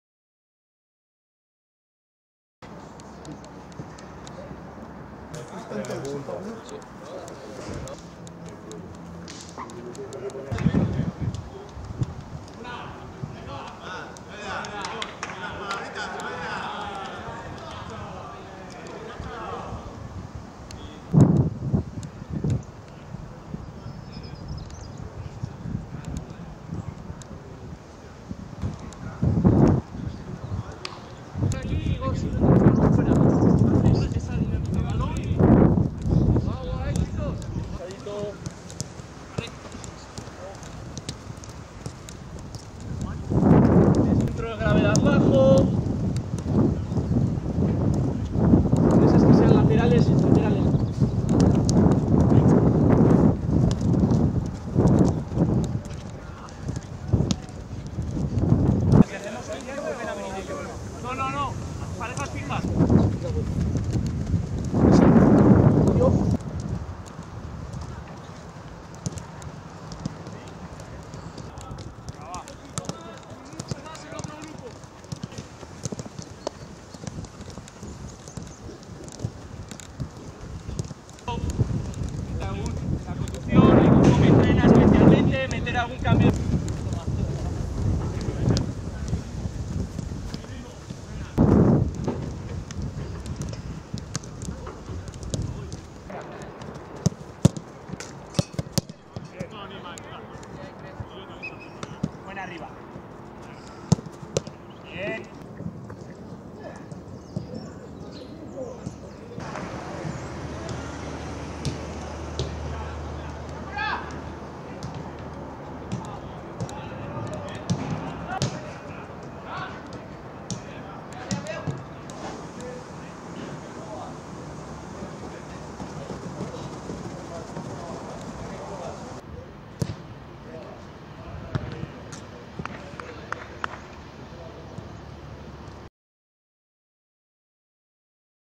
ENTRENAMIENTO | Preparando la visita a Nicosia | Real Sociedad
La Real Sociedad ya prepara el importante encuentro del próximo jueves en Nicosia ante el AC Omonoia.

Realak Zubietan bete du gaur goizean lan saio berri bat ostegunean Nikosian AC Omonoiaren aurka jokatuko duen partidari begira.